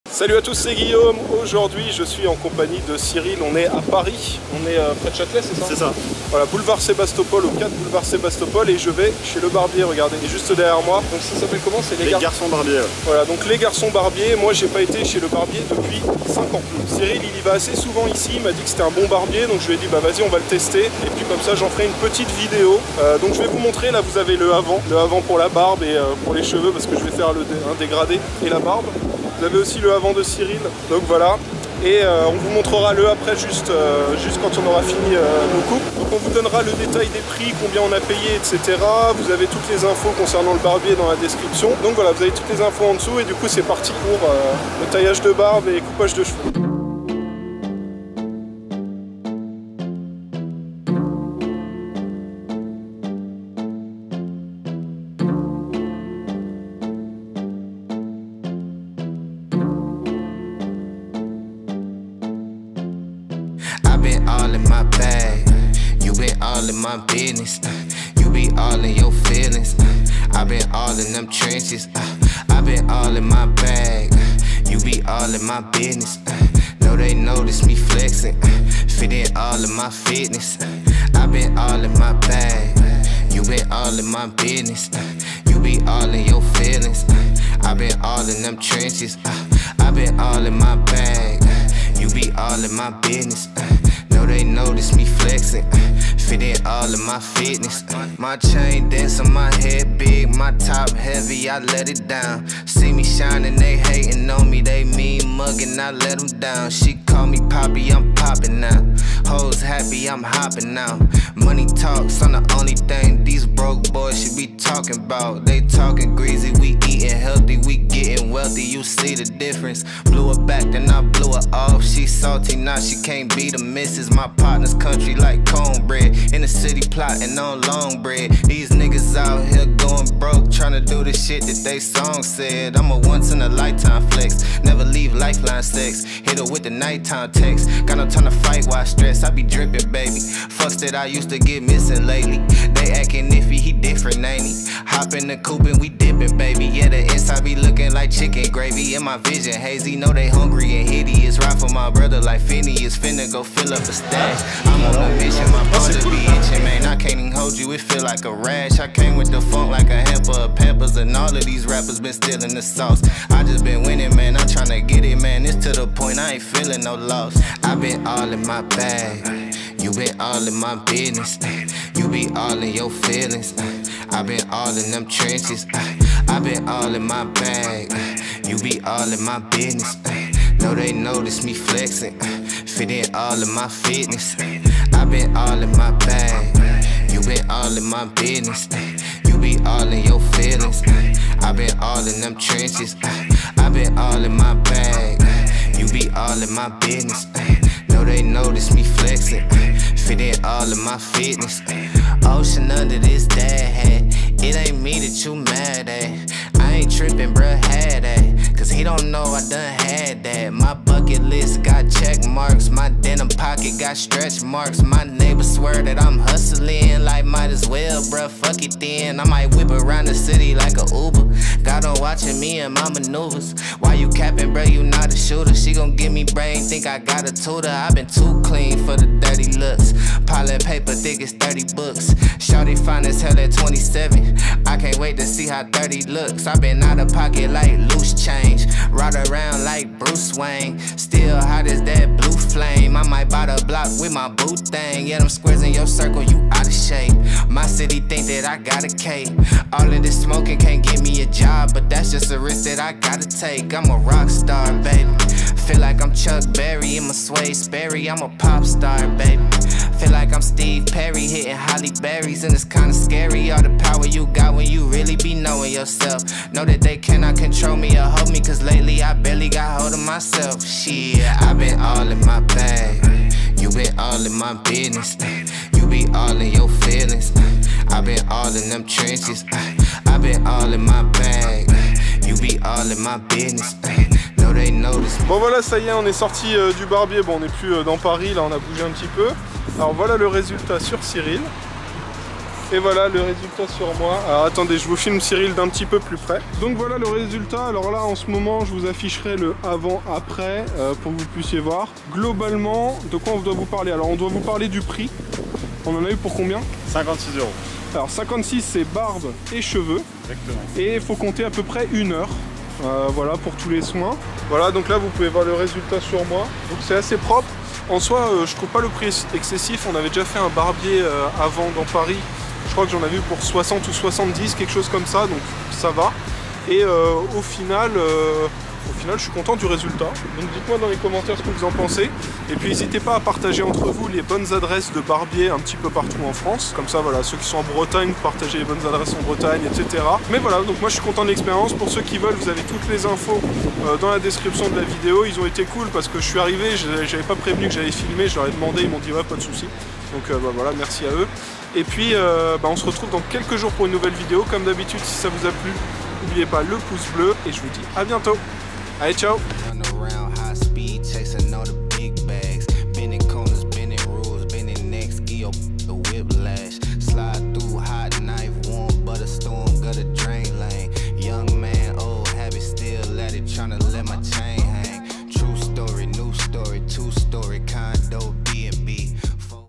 Je teste un BARBIER à PARIS
On est parti avec Cyril tester un barbier à Paris ! On est allés chez le barbier habituel de Cyril : Les Garçons Barbiers au 4 Boulevard de Sébastopol ! Bon visionnage :

Les Garçons Barbiers
4 Bd de Sébastopol, 75004 Paris